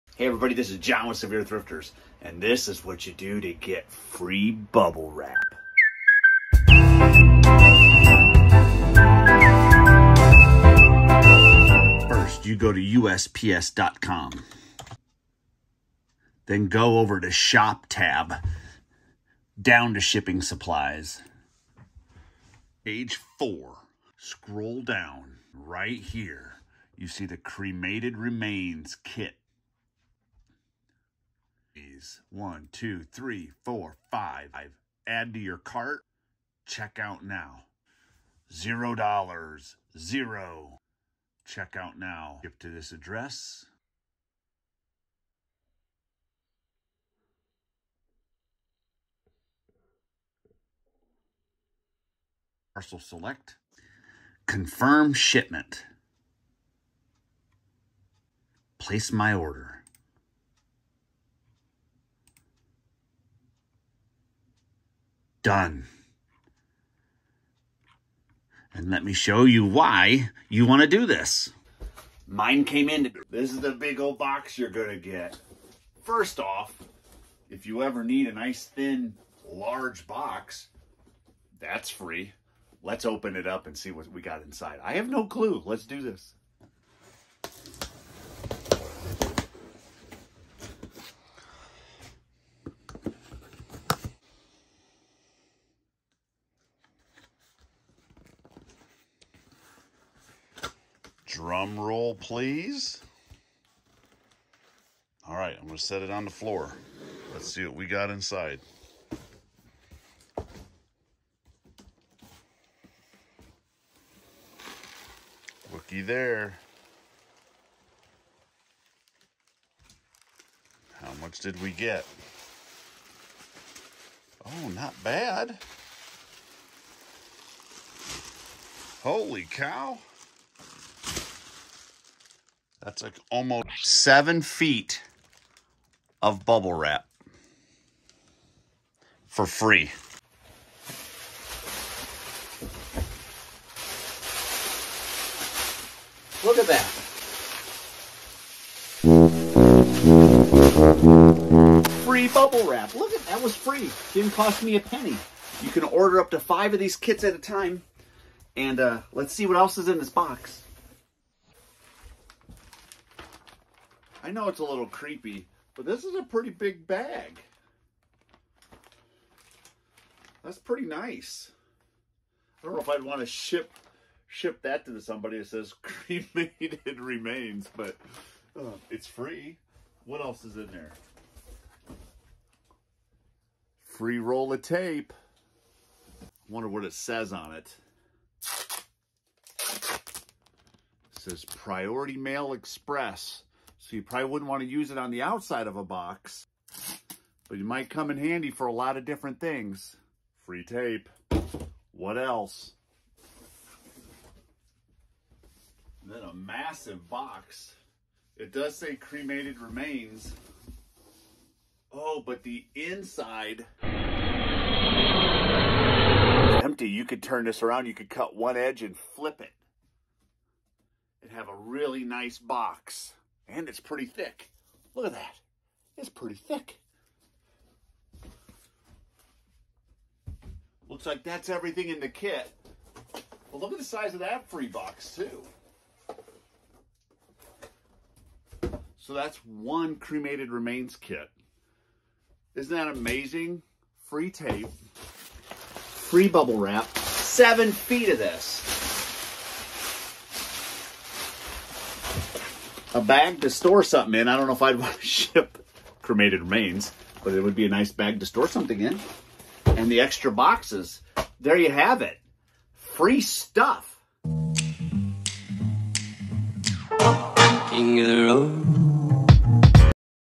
How to get FREE Bubble Wrap & Packaging Tape‼️
With rising shipping costs I am trying to find ways to cut my shipping supply costs!
If you enjoy Podcasts then check us out on Spotify or other Podcast Apps.  Just search SEVIER THRIFTERS
1. Go to USPS.com
2. Click the “Shop” tab
3. Click “Shipping Supplies”
4. Go to page 4
5. Click on the “Cremation Remains Kit”
6. Order up to 5 Kits at one time for FREE